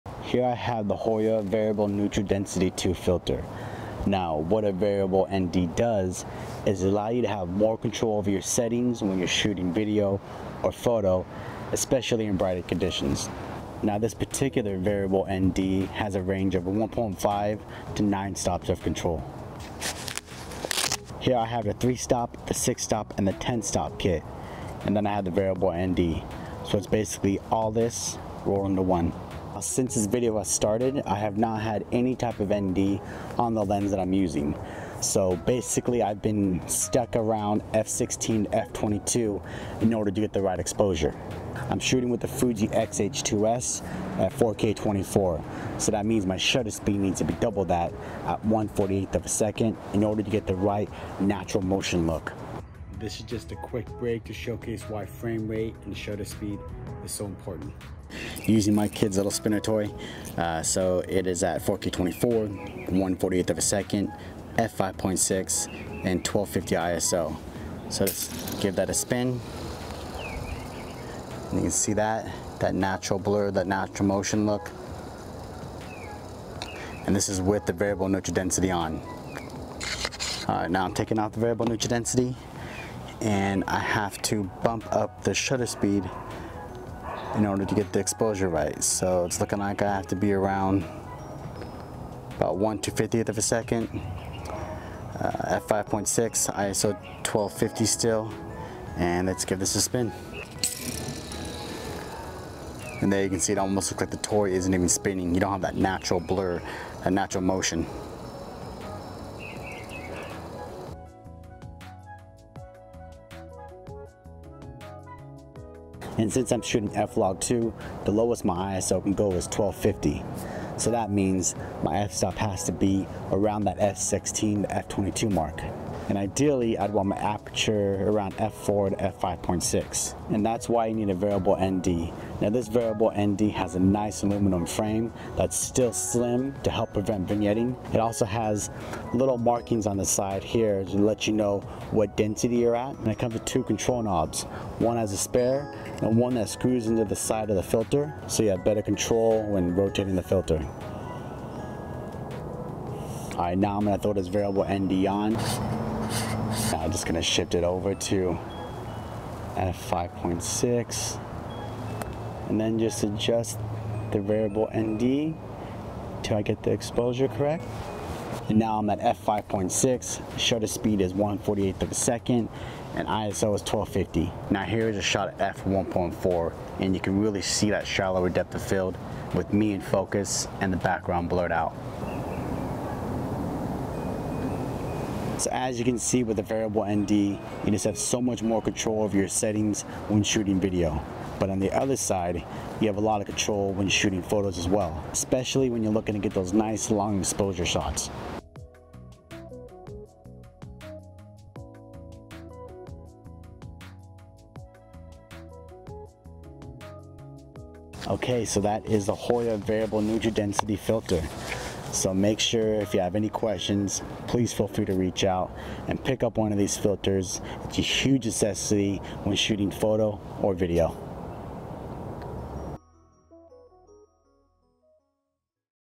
HOYA Variable Neutral Density Filter
A variable neutral density (ND) filter is a versatile tool for videographers and photographers, offering adjustable light reduction with a range of stops. It consists of two polarizing layers that rotate against each other, allowing precise control over exposure without changing aperture or shutter speed settings. This filter is essential for maintaining consistent exposure in bright conditions, enabling smooth motion blur in videos and creative effects like silky waterfalls in photos. Overall, it enhances creative possibilities by providing control over exposure settings in diverse lighting conditions.